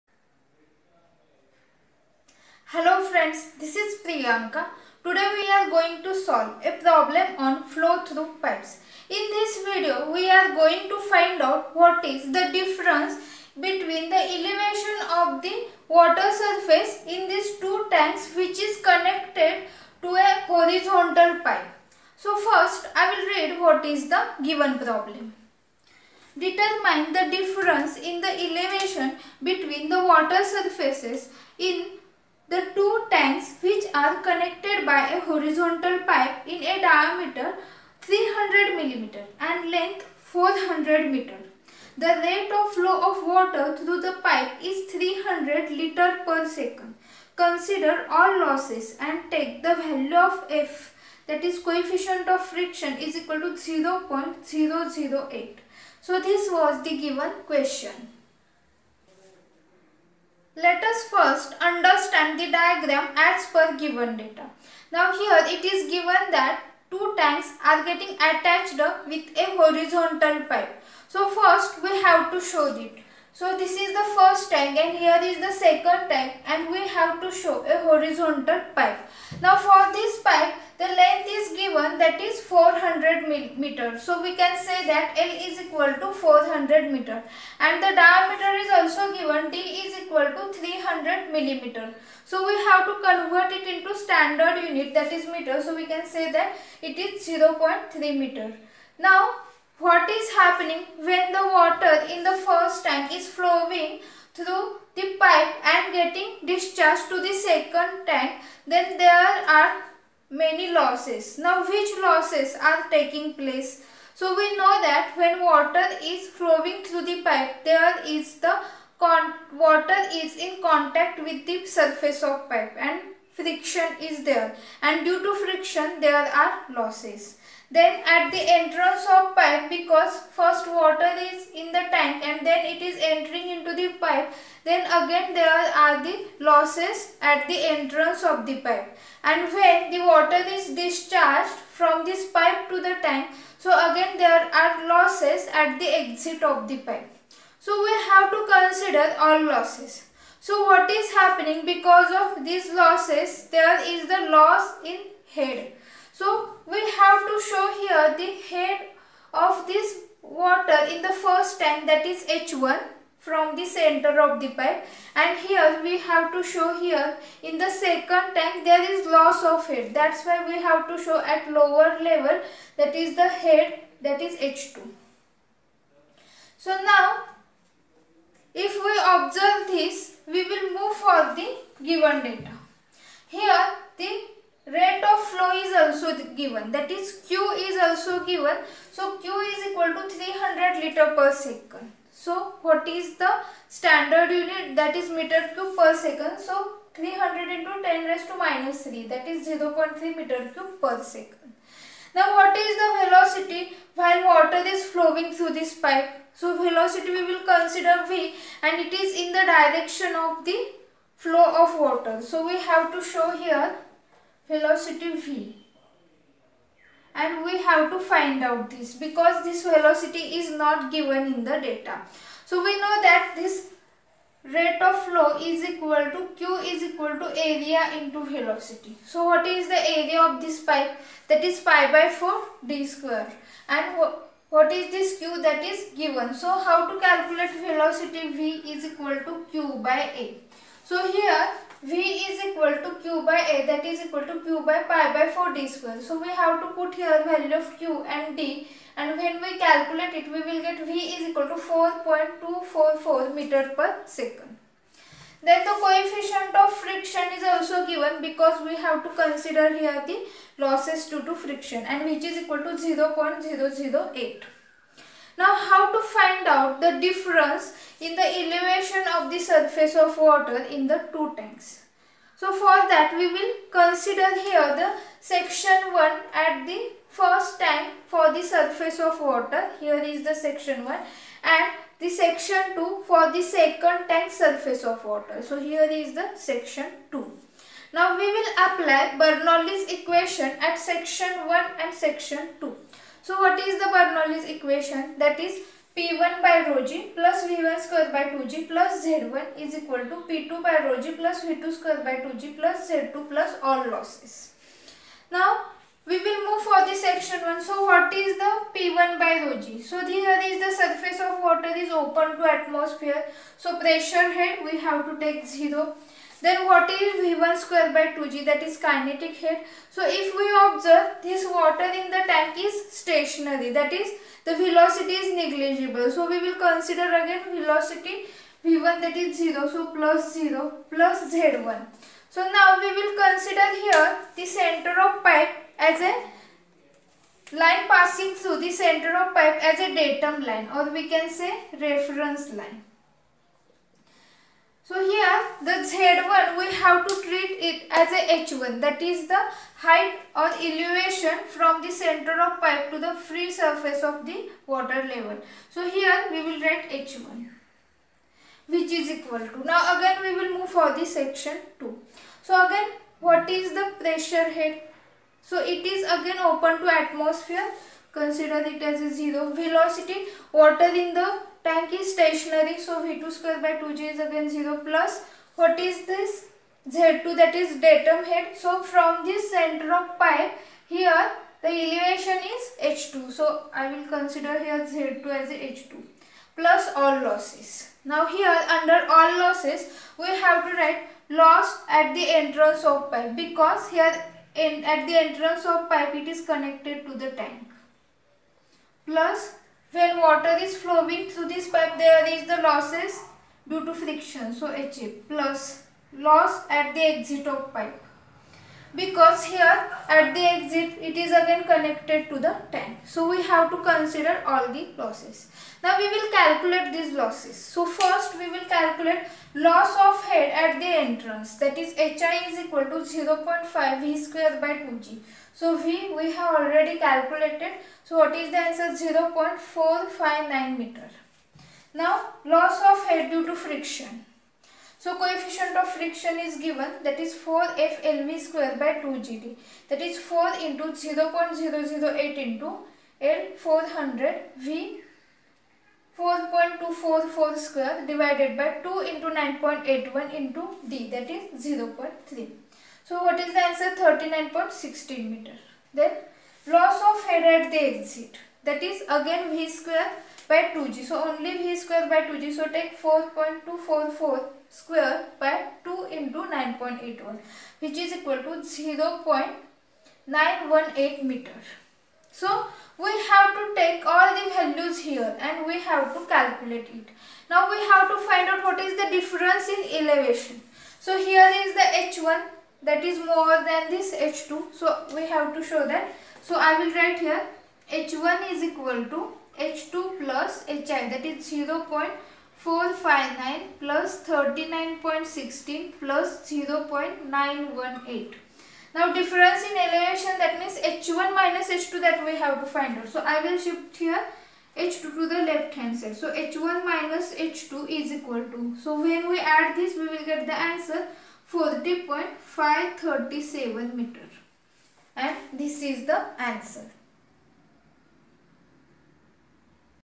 Problem on rate of flow through pipe attached with two tanks at ends major and minor losses
Determine the difference in the elevations between the water surface in the two tanks which are connected by horizontal pipe of diameter 300 mm and length 400 m. The rate of flow of water through the pipe is 300 litres/s . Consider all losses and take value of f= 0.008

My aim is to provide  guidance related to concepts of engineering topics